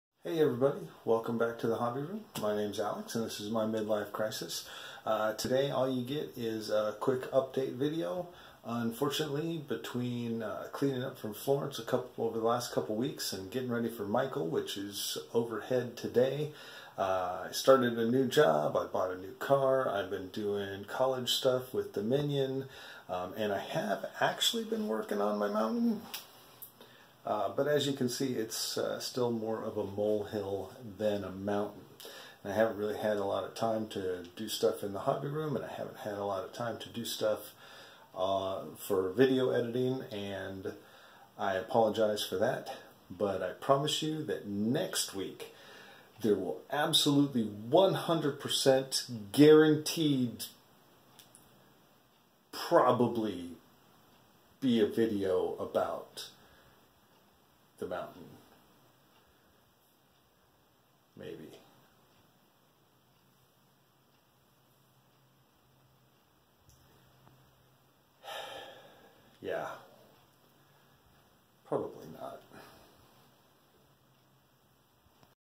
Just a quick update.
Wherein Alex explains that there won't be a video this week and makes a bunch of excuses